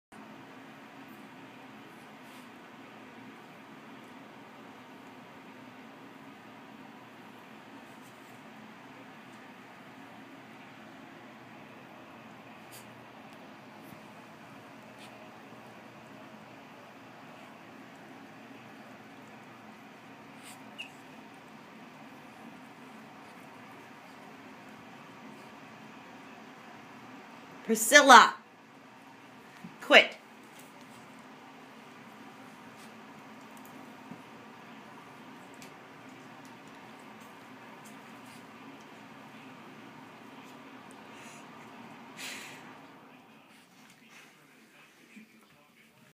Cat Cleans Dog. Dog's Sister moves in.
My cat Ollie was saved after being attacked by an animal. You would think he would not like other animals after that. But he loves his dogs. My poodle Blue wanted Ollie to clean her and he did.  Blue's sister, my other poodle Prissy, wanted Ollie to clean her instead. Enjoy!!! :)